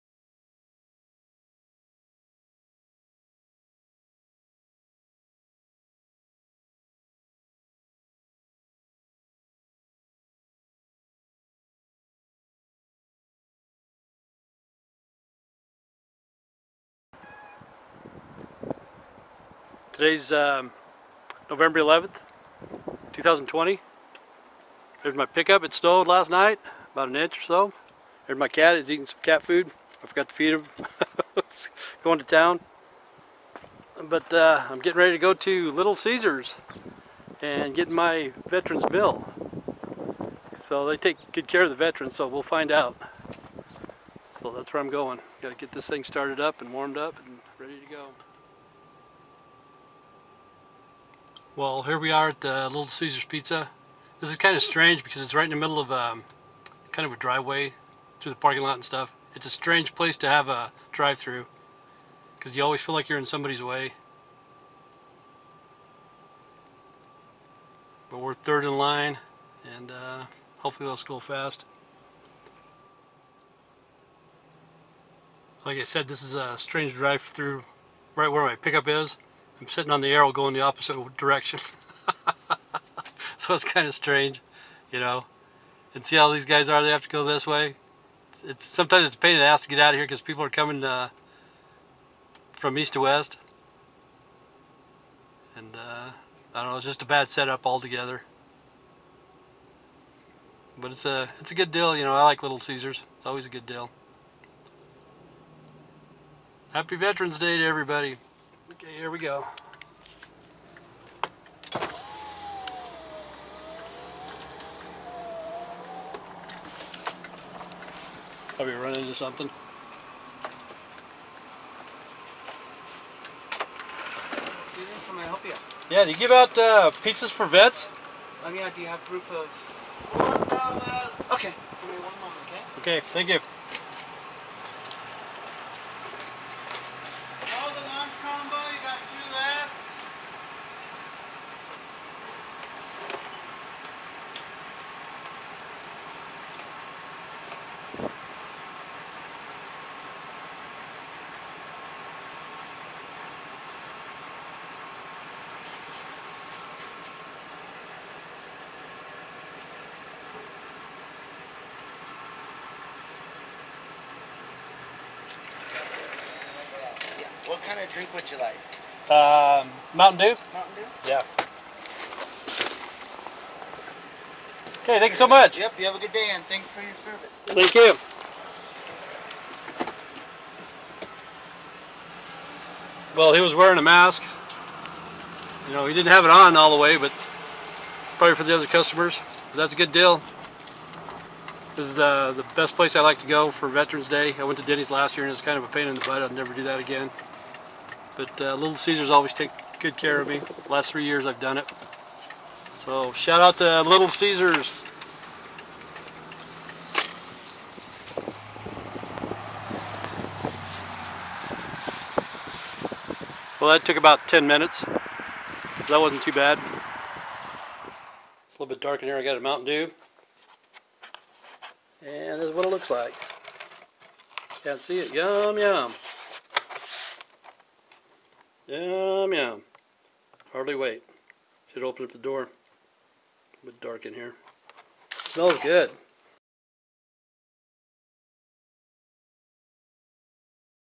Veterans Day Pizza
Little Caesars feed the VETERANS A MEAL ON  VETERAN'S DAY.  Pizza, soda pop lunch combo from 11a.m till 2p.m every Veteran's Day. A GREAT BIG SHUTOUT FOR LITTLE CEASARS.